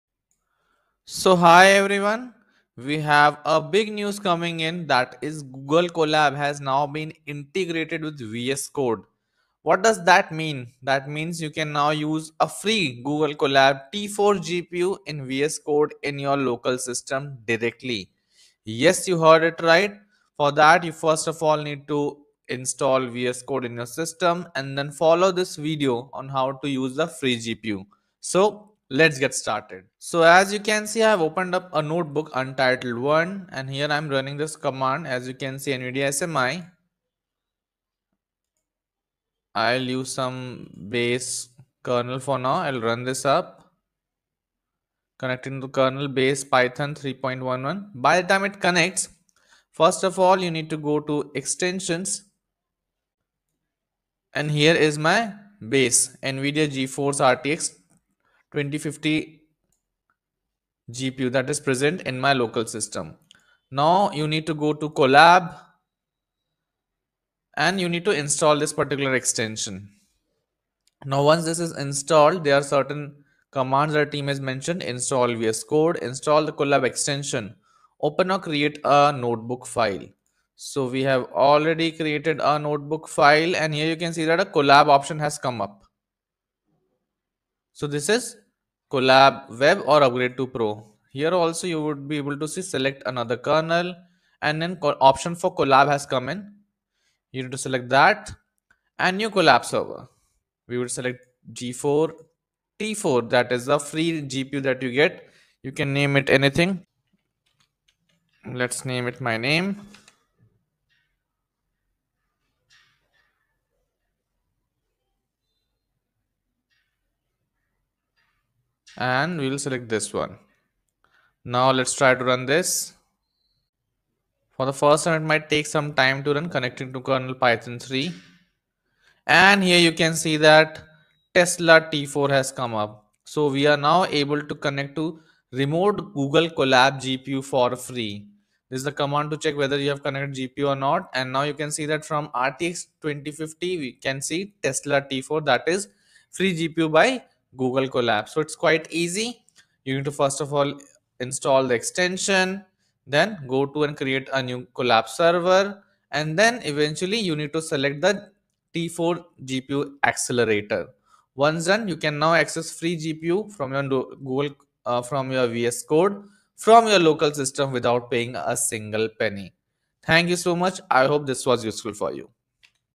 Free GPU using VS Code demo
Google Colab has now been integrated with VS Code. How to access free GPU from Google Colab using VS Code